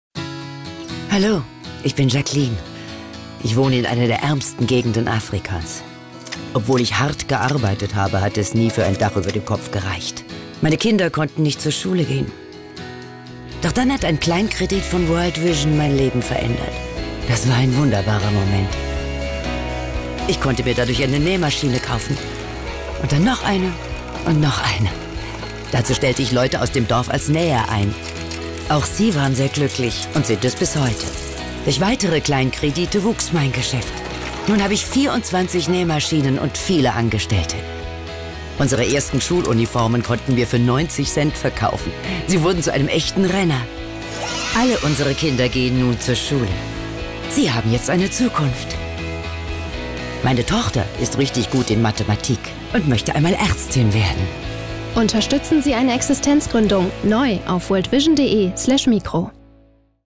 So erfolgreich unterstützt Micro - der "Gute Kredit" Jacqueline in Afrika:
Mit einer Spende ab 25 Euro tragen Sie dazu bei, die benötigte Kreditsumme zu erreichen und ermöglichen einem Existenzgründer sein eigenes Geschäft aufzubauen oder zu erweitern